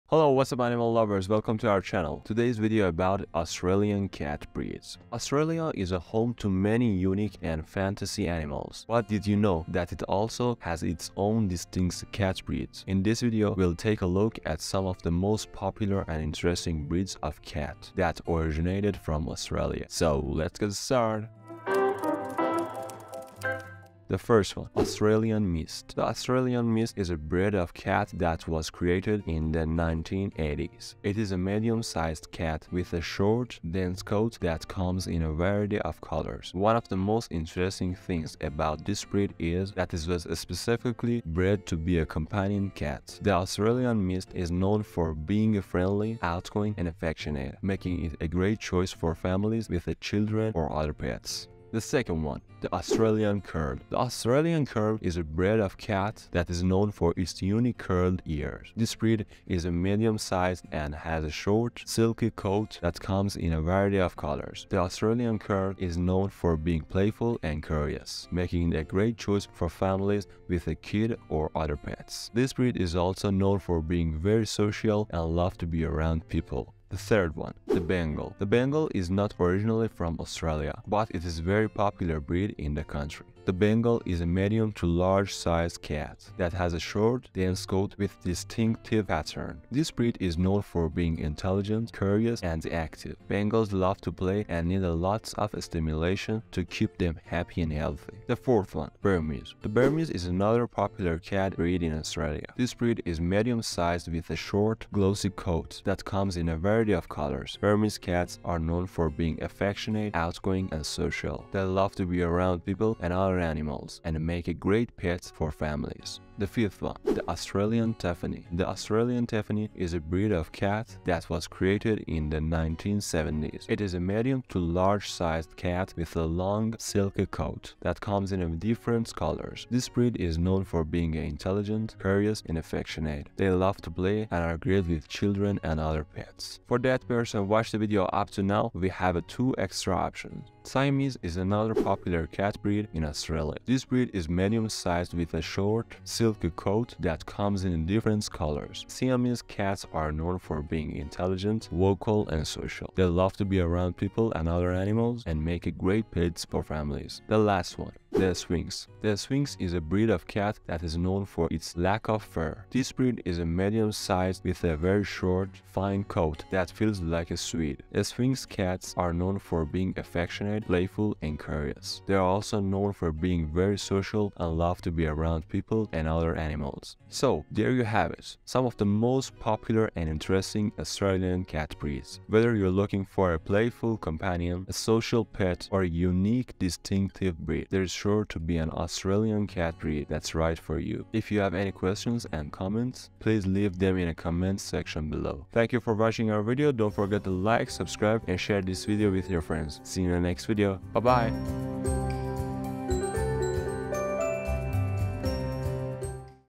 Top 5 popular Australian cats breed
In this video, we'll be showcasing the most beloved and sought-after cat breeds down under. Whether you're a cat lover or just looking for your perfect feline companion, this video will give you an insight into the top Australian cat breeds.

Time codes
0:00 - Intro
0:24 - First
0:51 - Secend
1:17 - Third
1:38 - Fourth
1:56 - Fifth
2:20 - Extra
2:36 - Extra
2:57 - Important Point

Whether you're looking for a playful, outgoing feline friend or a laid-back lap cat, our list has something for everyone. And with their stunning looks and captivating personalities, these breeds are sure to steal your heart.

So sit back, relax, and join us as we count down the Top 5 most popular cat breeds in Australia.